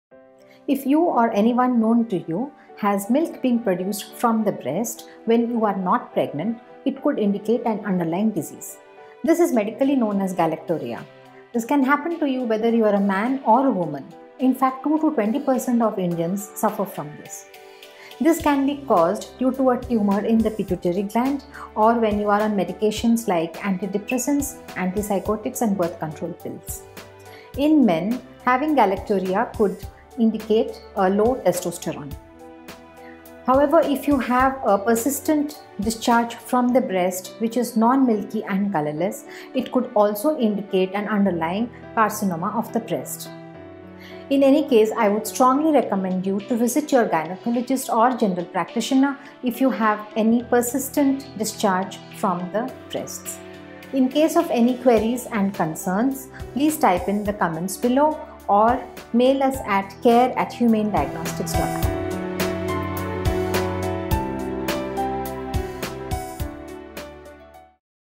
Why Do Some Men Secrete Breast Milk? | Humain Diagnostics
Breast milk in men is related to Gynaecomastia. This condition is called Galactorrhea.

In women, if breast milk is produced without a pregnancy, it is not a healthy sign.

About 2-20% of Indian women can experience breast milk production without pregnancy.

Galactorrhea is a documented medical condition, that affects both sexes, and is a sign of serious health diseases.

To help you understand this complex disease, Dr. Leena Appicatlaa, Humain's Senior Medical Director, explains what you need to look out for.

Follow Humain Diagnostics, India's First AI-powered lab chain, for everything health and wellness.